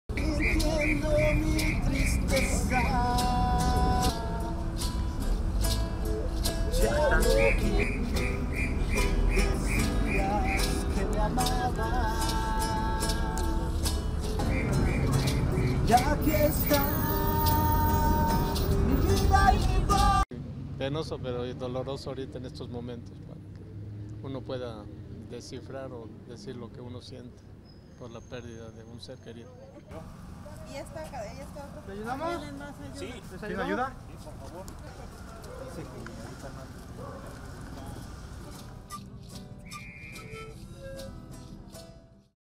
Mourners lay flowers for young victims of Mexico quake
Relatives, friends and neighbours laid flowers in memory of the children who were killed when a 7.1-magnitude quake toppled an elementary school in Mexico City.